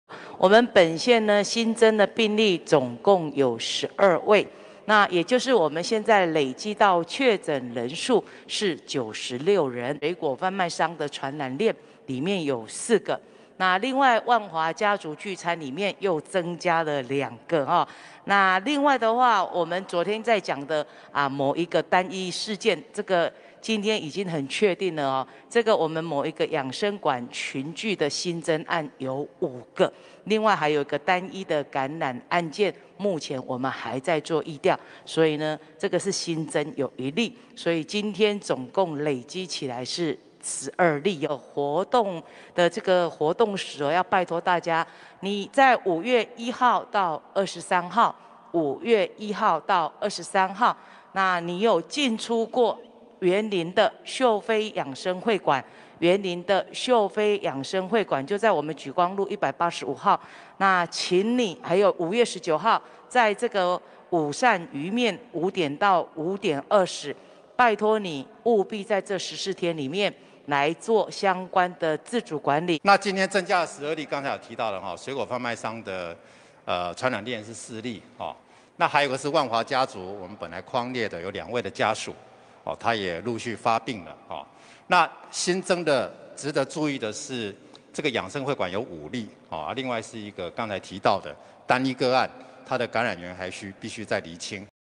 彰化增新增傳染鏈為一家養身會館5人確診
彰化縣長王惠美和彰化縣衛生局長葉彥伯今公布全縣武漢肺炎確診個案新增12例，新增在養生會館工作的單一個，5名同事檢驗結果全都中鏢